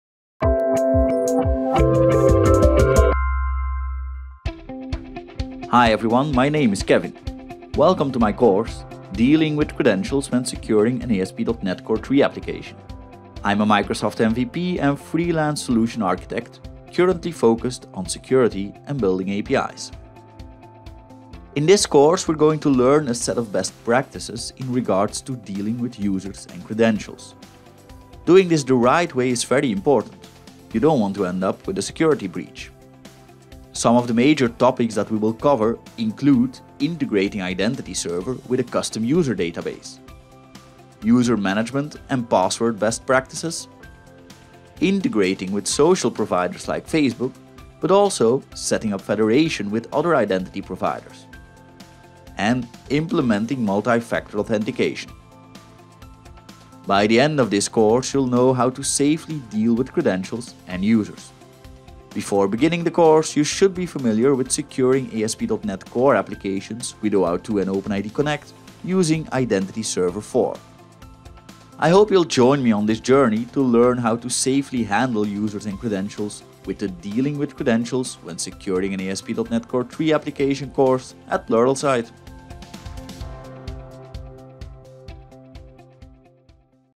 Dealing with Credentials When Securing an ASP.NET Core 3 Application Course Preview
Join Pluralsight author Kevin Dockx as he walks you through a preview of his "Dealing with Credentials When Securing an ASP.NET Core 3 Application" course found only on Pluralsight.com. Build your technology skills with Kevin’s help by learning best practices in regards to dealing with users and credentials, how to integrate IdentityServer with a custom user database, and much more. Visit Pluralsight.com to start learning ASP.NET Core 3 skills today!